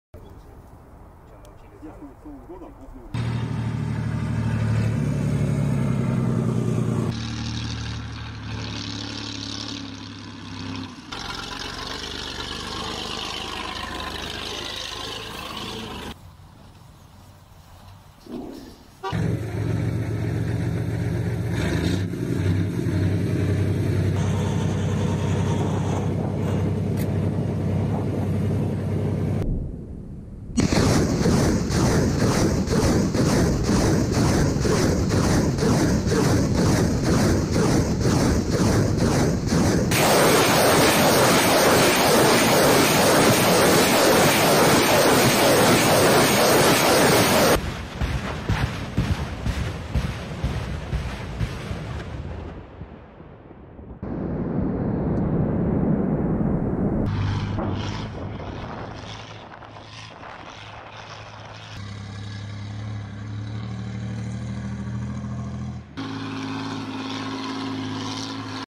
Russia claims to have destroyed American M777 Howitzer using 'Grad' MLRS
Rusia telah mengklaim bahwa mereka mengeluarkan kendaraan amunisi howitzer M777 Amerika dengan rudal yang ditembakkan dari sistem peluncuran roket ganda 'Grad'.

Rekaman dimulai dengan menunjukkan sistem peluncuran roket ganda (MLRS) BM-21 Rusia 'Grad' bermanuver ke posisinya sebelum terlihat menembakkan rudal ke langit, melaporkan menargetkan target Ukraina yang tak terlihat.

Kendaraan kemudian terlihat bermanuver menjauh dari area tersebut untuk menghindari balasan tembakan.